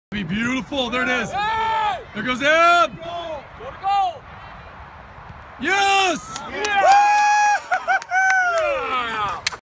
Abby Hazen Goal Body off Defender 6/24/2017
Pacific SC Abby Hazen, #5 (Blue) Position 7 (right wing)
Opponent: Lazers SC G01 CSL Silver - Shadrick
Date: 6/24/2017
PSC W 4-0
Abby Hazen 2 goals / 8 shots / 4 crosses / 48 mins
Video: Beating Mark and Cross
2017 Surf City Cup - Pool Play